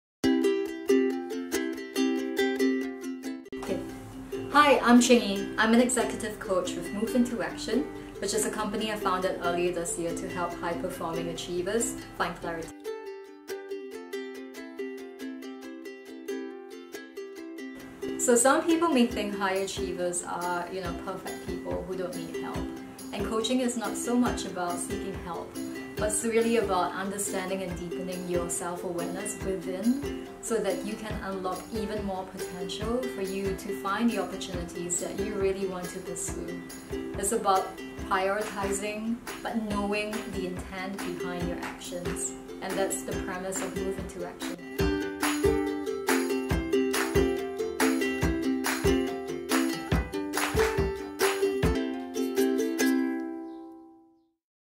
I'm an achiever. Do I really need coaching?
We speak to Ching of Move into Action to understand why coaching can be beneficial to even high achievers and performers.

About Move Into Action:
It is a coaching service for professionals looking to strengthen their personal brand, communication skills and leadership style.